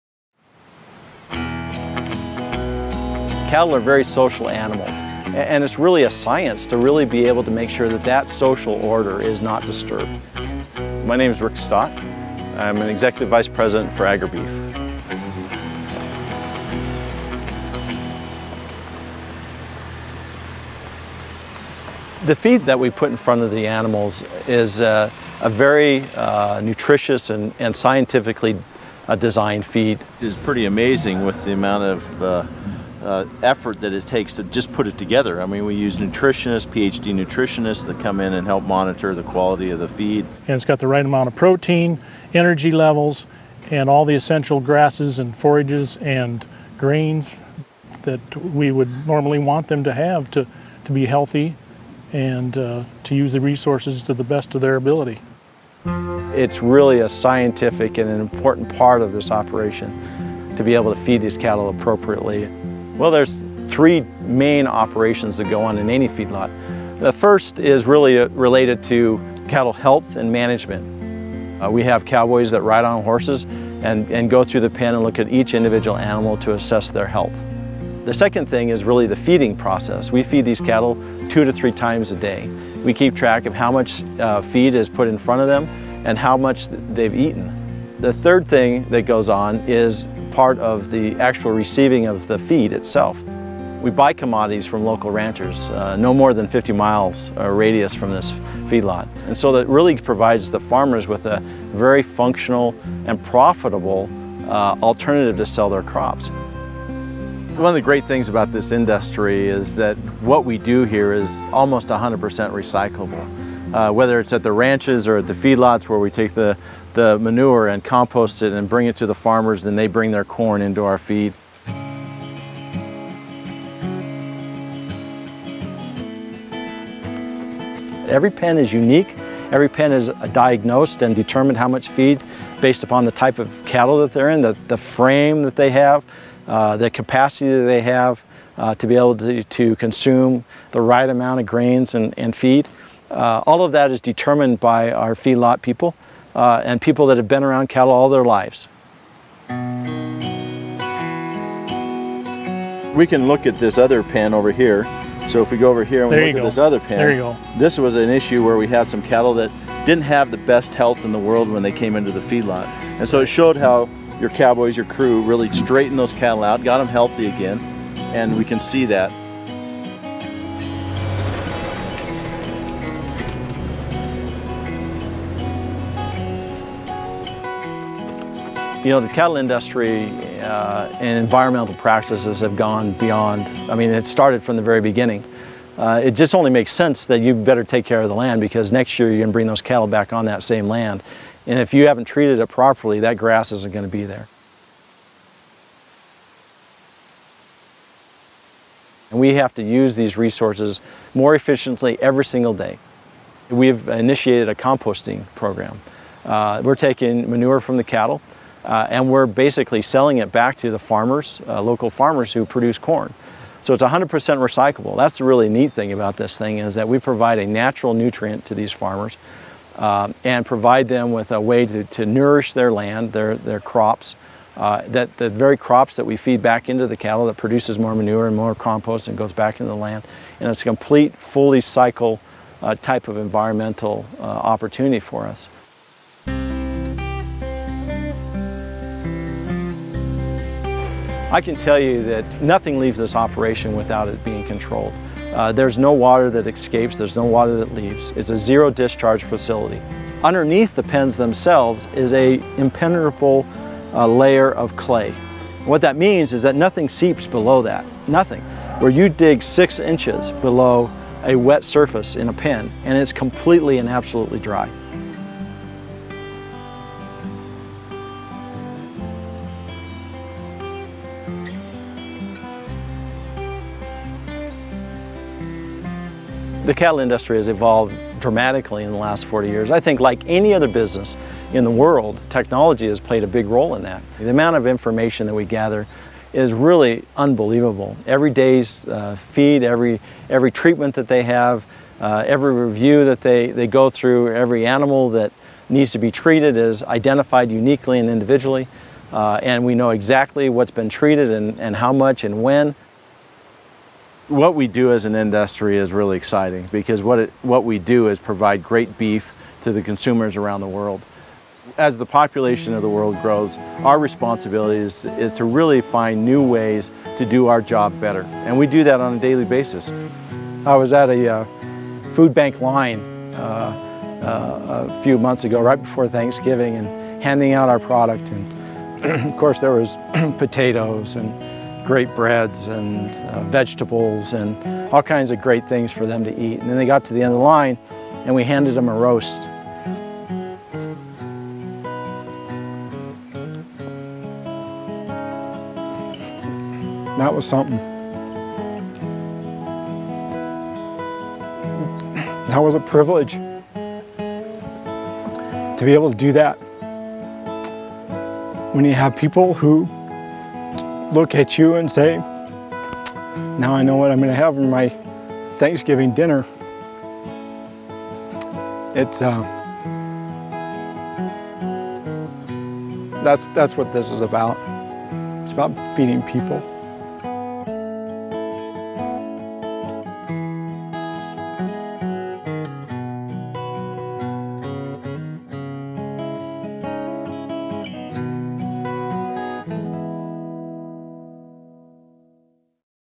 Beef Documentary
An emotional portrayal of farmers discussing the science behind a cattle feedlot and the care involved in raising the cows. The environmental practices they observe and are constantly improving upon are showcased.
For more information and to join the conversations, please visit us